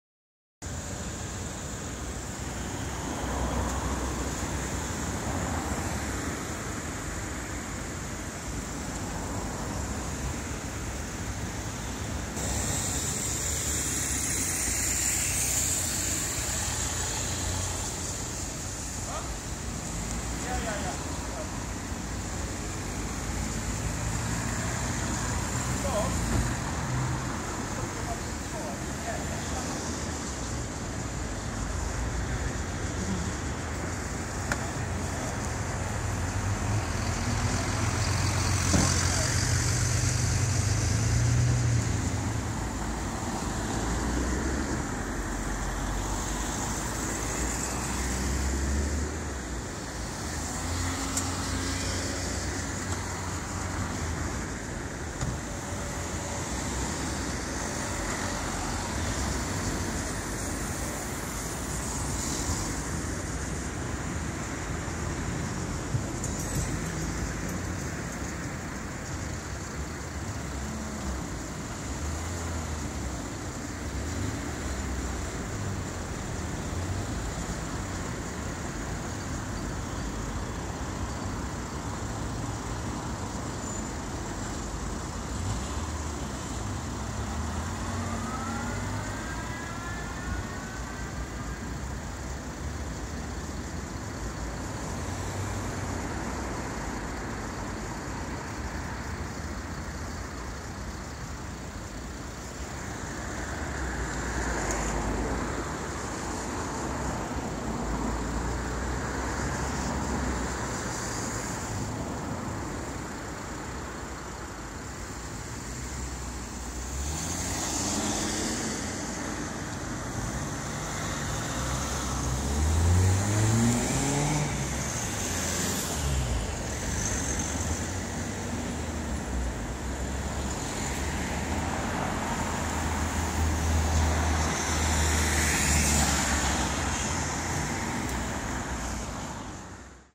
Gloucestershire Constabulary - VW Golf Boxed in by X5 ARV, Astra Estate IRV and a Sprinter PO Van
If any of my information is incorrect, notify me and I will change it accordingly.

Sorry about the quality, I had to record it on my phone.

Service: Gloucestershire Constabulary
Area: Gloucester
Make: N/A
Model: N/A
Year: N/A
Plate: N/A
Type: N/A
Force/Organisation: Gloucestershire Constabulary